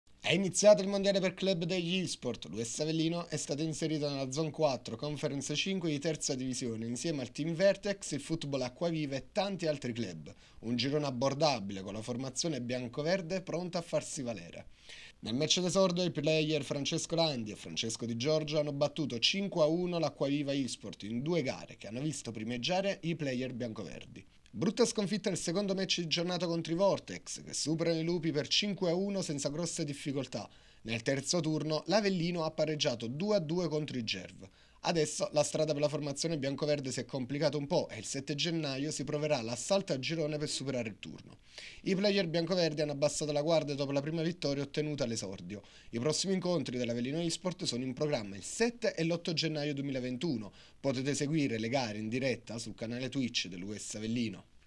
Avellino eSports: esordio con vittoria al Mondiale per club, battuto 5-1 l'Acquaviva
È iniziato il mondiale per club degli Esports.